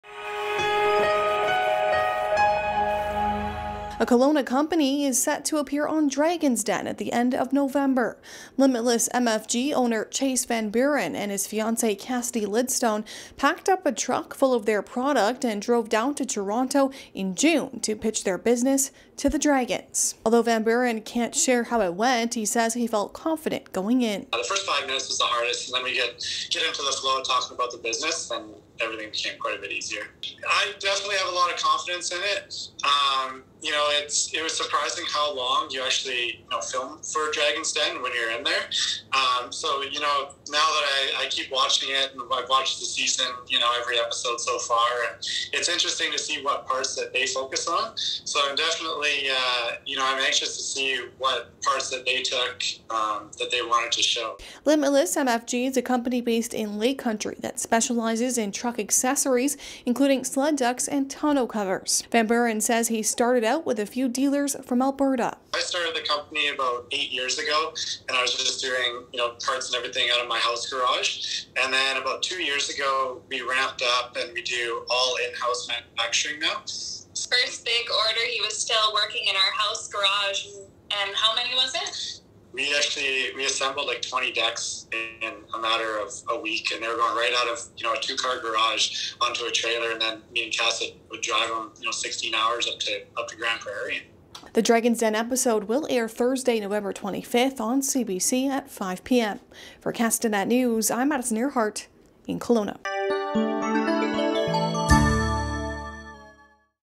Locals on Dragons' Den
A Kelowna company is set to appear on Dragons' Den at the end of November.